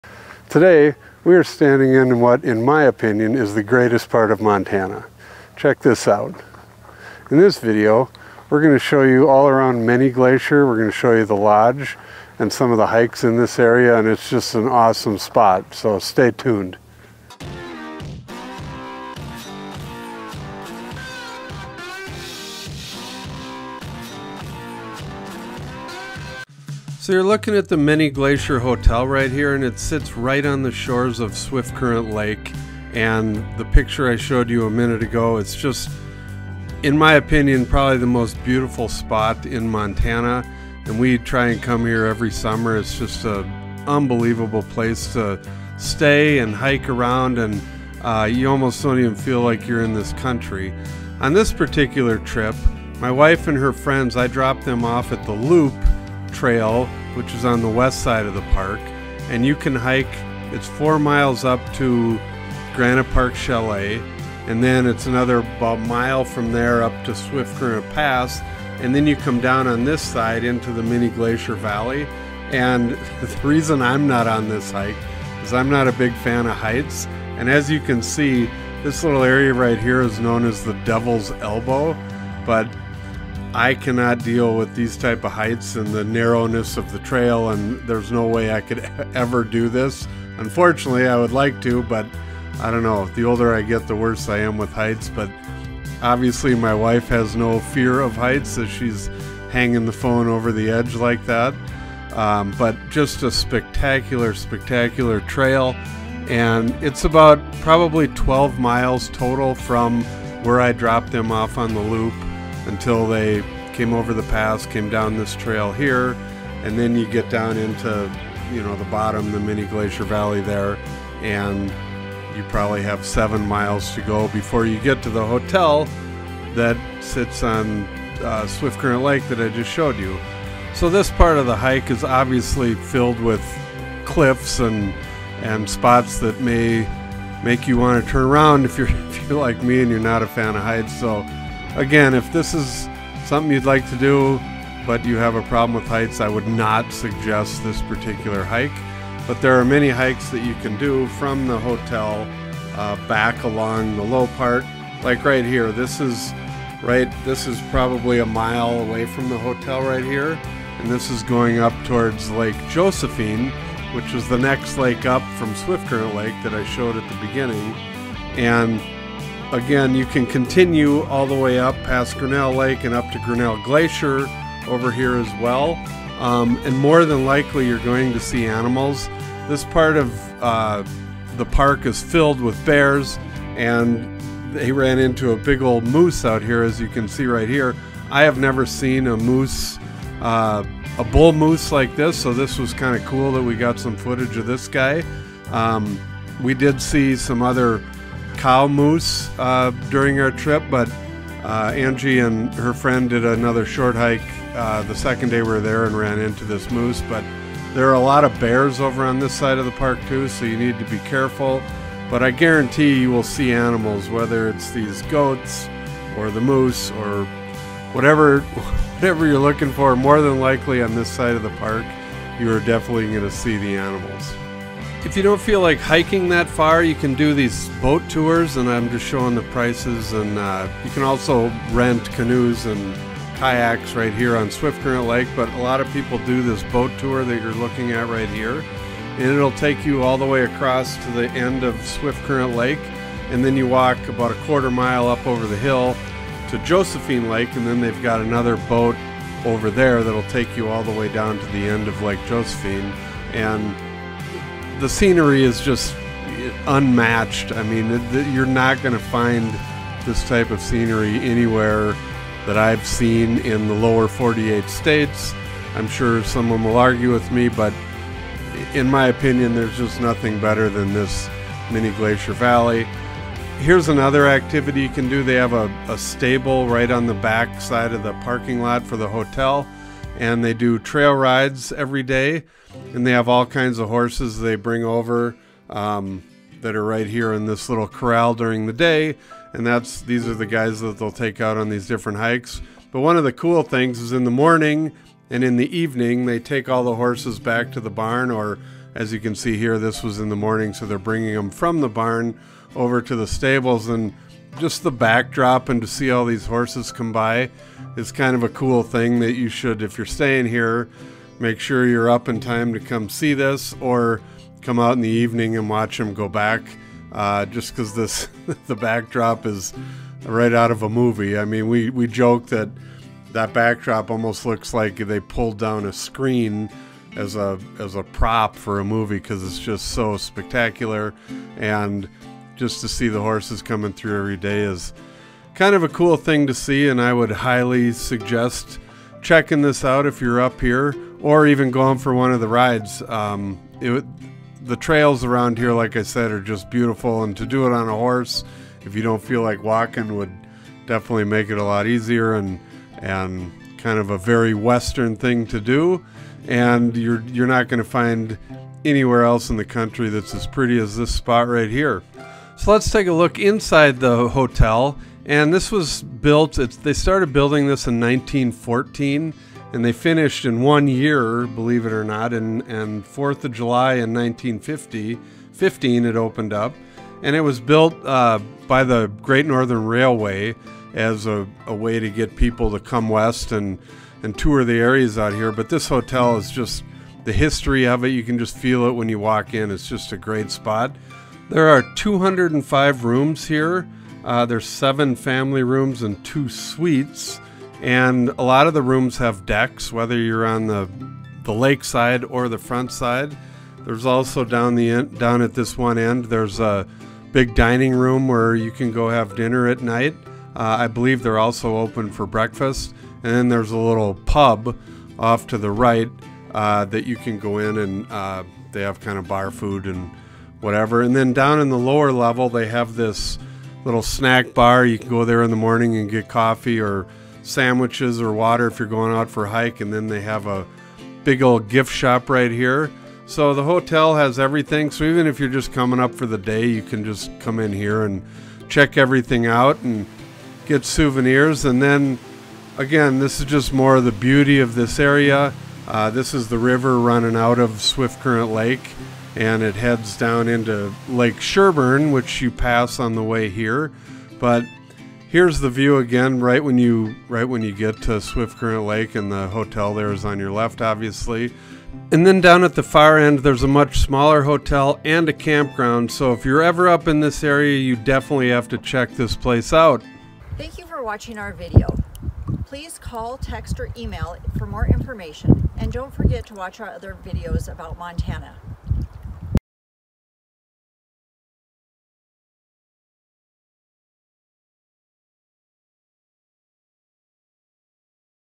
The Most Beautiful Spot in Glacier Park
The Many Glacier Valley is one of the most beautiful spots in Montana! Located on the east side of Glacier National Park, this is a must-see if you are headed to the park. In this video, we will show you around the Many Glacier Valley and look at the activities that are available in the area. Boating, horseback riding, and hiking are some of the great ways to explore this area. We will also tour the famous Many Glacier Hotel. If you are planning a visit to Montana, put this spot on your list!

0:00 Many Glacier
0:50 Hiking around Many Glacier
3:00 Animals in Many Glacier
3:43 Boat tours in Many Glacier
4:32 Horseback riding in Many Glacier
6:00 Many Glacier Hotel

Will Friedner
Hillary Vrana
Angie Friedner

Get Your Montana Life Swag Here: Montana Life Hoodies and Shirts